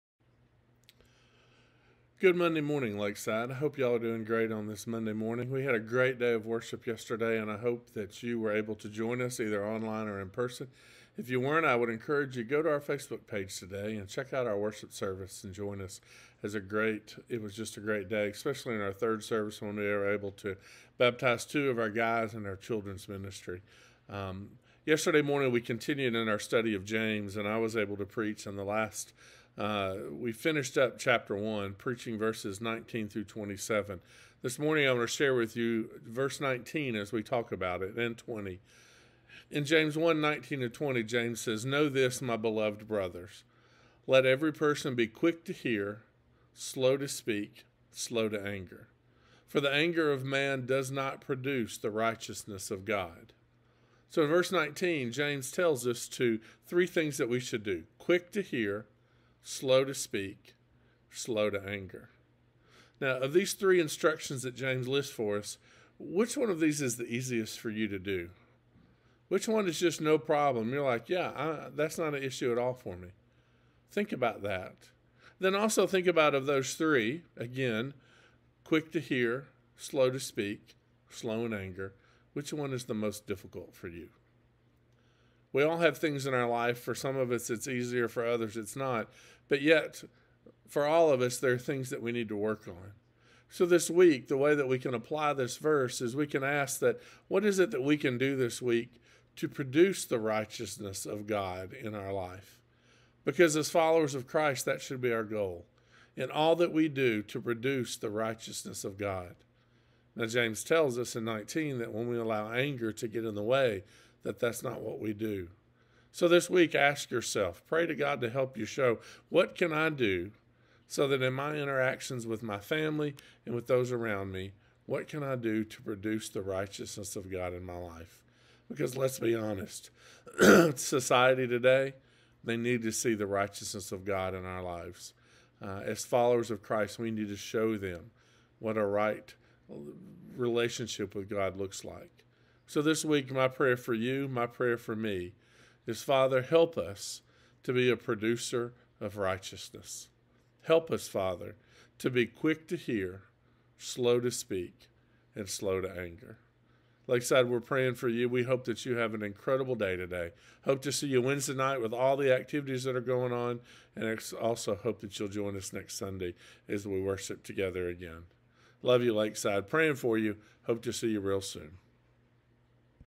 SOAP February 15, 2021
S - Scripture; O - Observation; A - Application; P - Prayer

S - James 1:19-20

“Know this, my beloved brothers: let every person be quick to hear, slow to speak, slow to anger; for the anger of man does not produce the righteousness of God.”

 O - Which of the instructions that James shares in verse 19 is the most difficult one for you to do? Which is the easiest?

A - What can you do this week to produce the righteousness of God in your life?

P - Father, help me be a producer of righteousness; help me to be slow to anger.